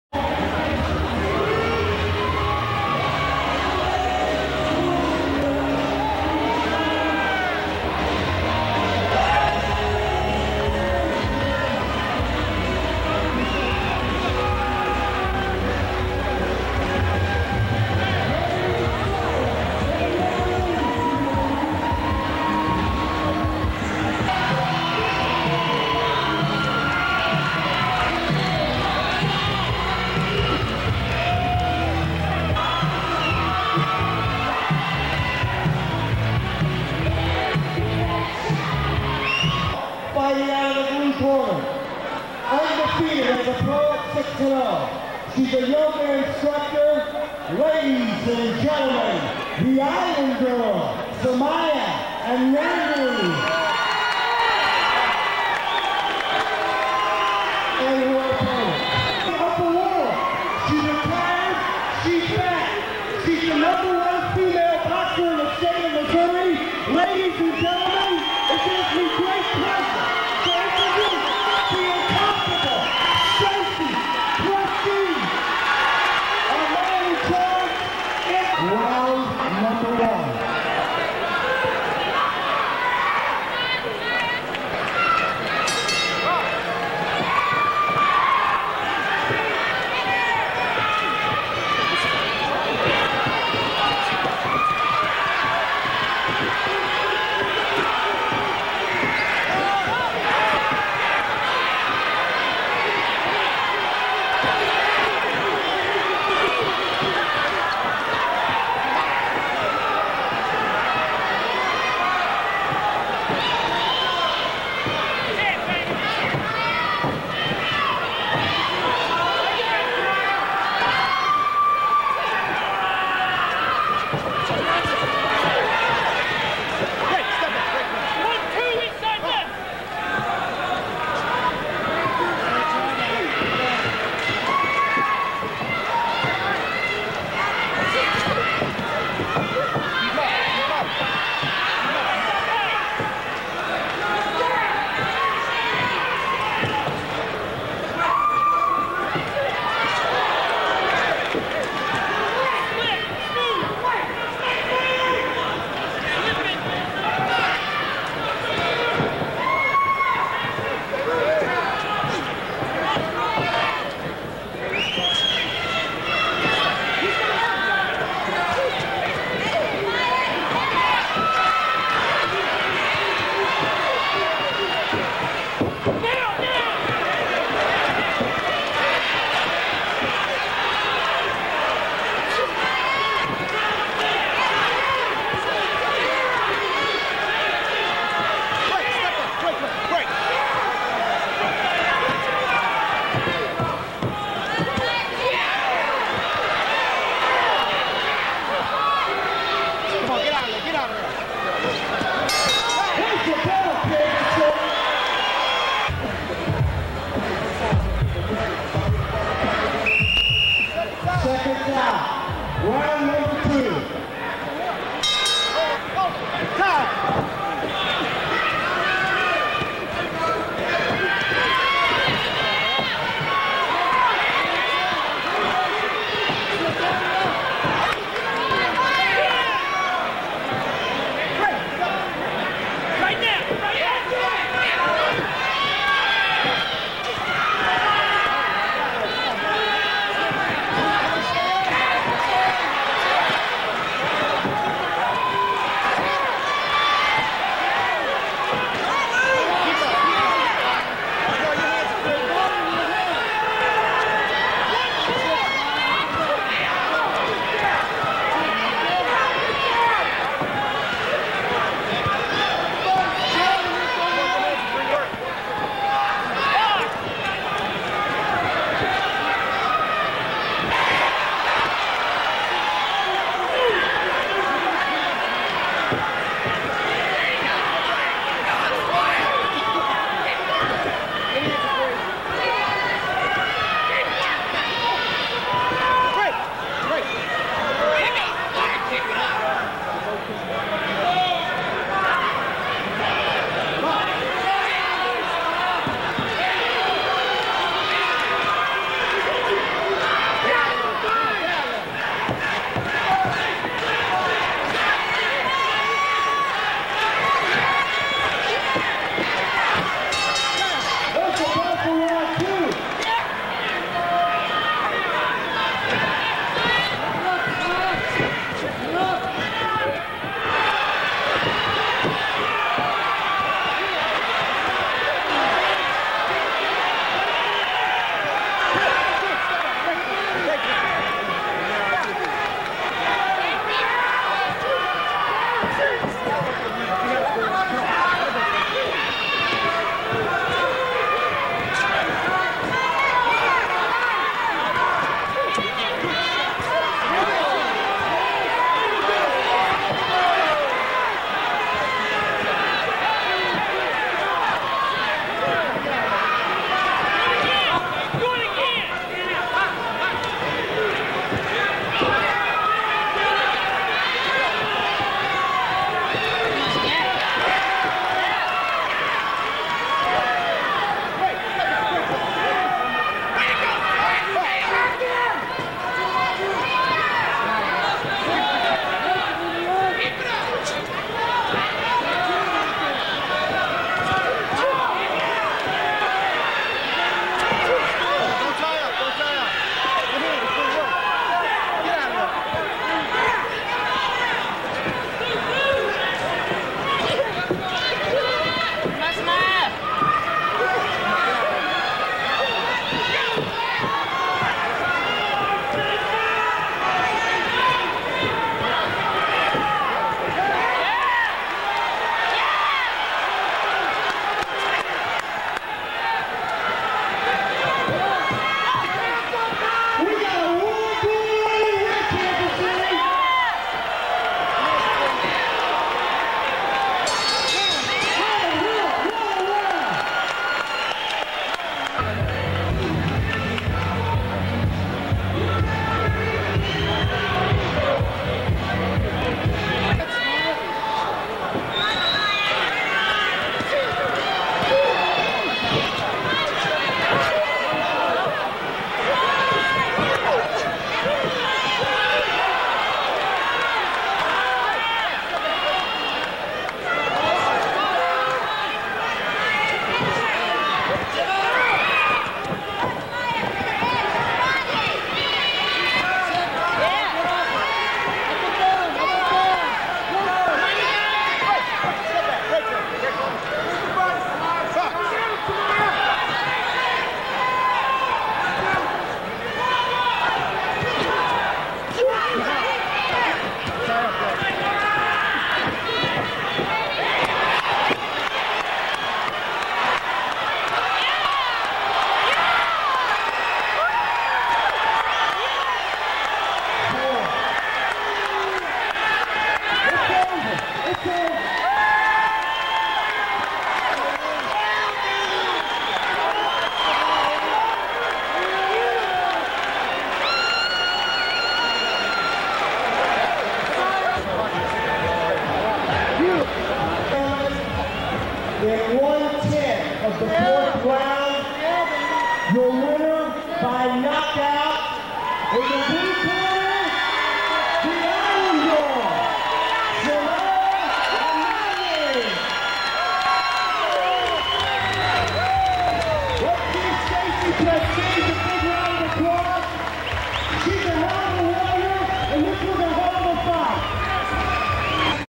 Sumya Anani vs Stacey Prestage women's boxing match Sep 30, 1997
Held at the KC Market Center, Kansas City, Missouri, USA. Sumya was 6-0 going into the fight.